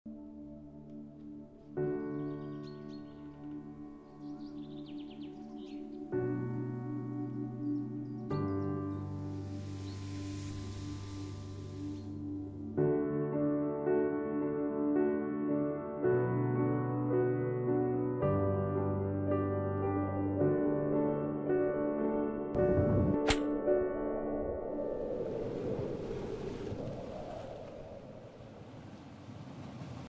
Experience The New Standard of Luxury in the Heart of the Dominican Republic
Casa de Campo, nestled on the stunning southeastern coast of the Dominican Republic, has always been synonymous with opulence and exclusivity. We're thrilled to showcase the resort's latest offerings and its expansive, lush property that spans over 7,000 acres of pure bliss.